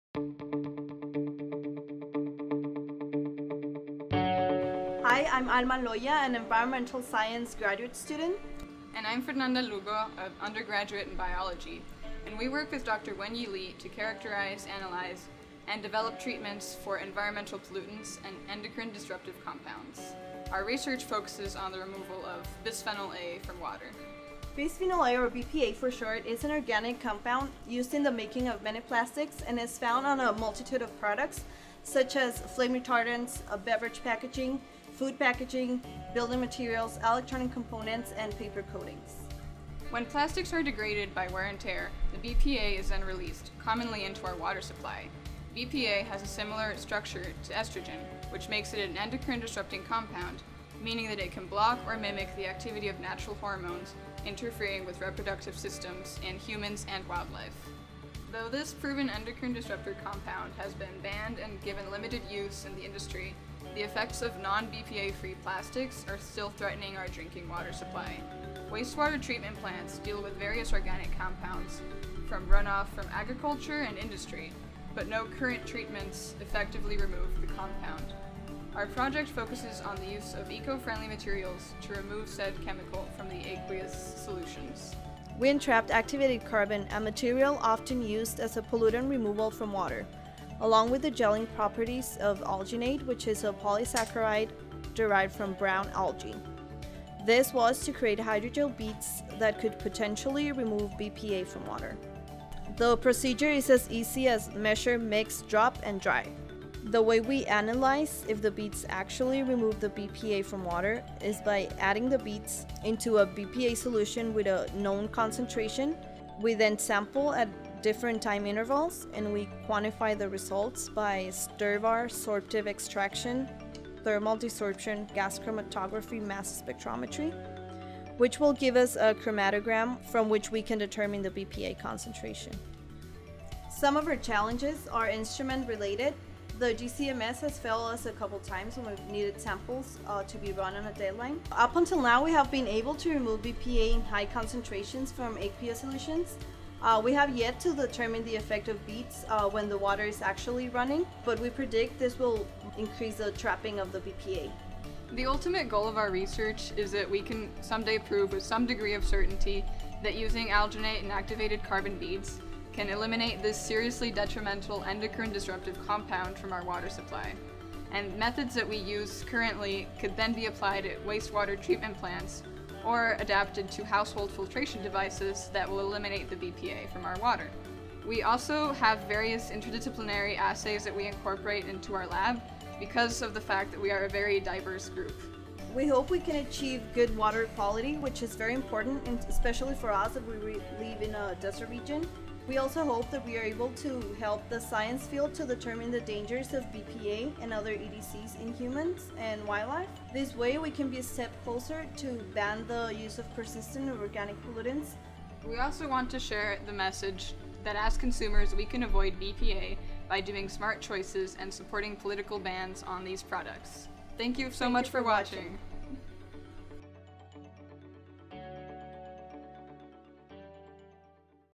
UTEP Department of Chemistry: Bisphenol A Removal with ACA Beads (April 2017)
Bisphenol A (BPA for short) is an organic compound used in the making of many plastics and is found in a multitude of products including food and beverage packaging, flame retardants, adhesives, building materials, electronic components, and paper coatings.

Our research focuses on the removal of Bisphenol A from water.
how the ACA beads effectively removes BPA is by sampling a solution treated with beads over time and analyzing samples at differing concentrations of BPA by use of Gas Chromatography and mass spectrometry, which basically tells us the concentration in the sample.

Research conducted by Dr. Wen Yee Lee’s Environmental Chemistry Lab at UTEP. Our research analyzes, characterize analyze and develop treatments for environmental pollutants and endocrine disruptive compounds.

Video produced by Creative Studios @ The University of Texas at El Paso.